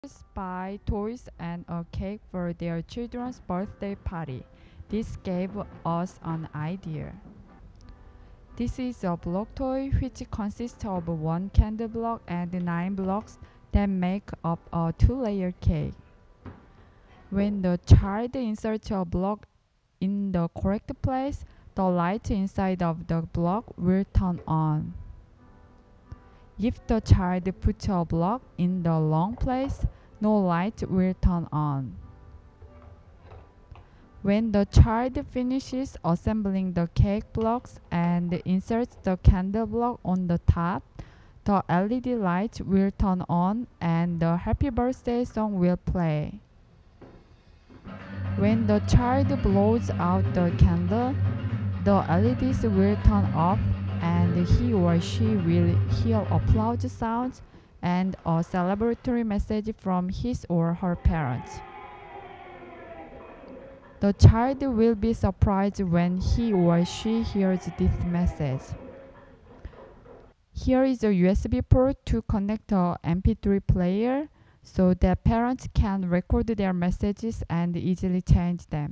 Interactive Birthday Cake
Demonstration movie of interactive birthday cake for Maker Faire 2007